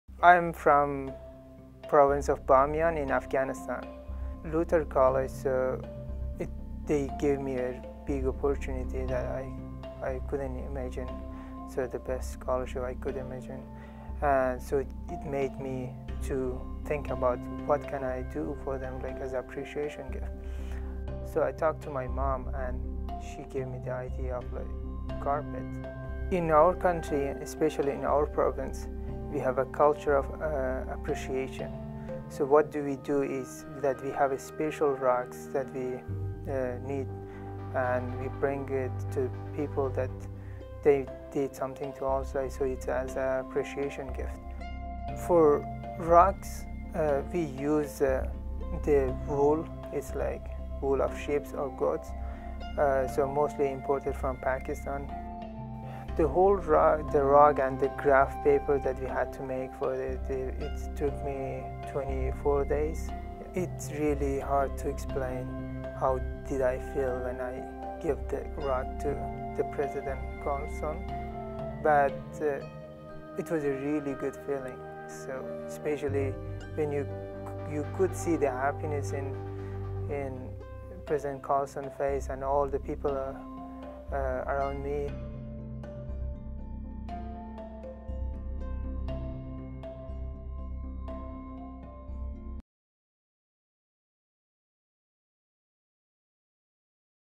Luther College - A Gift of Appreciation
Student Ali Yazdani created a rug to give to Luther College as a gift of gratitude. Special thanks to the Luther College Diversity Center.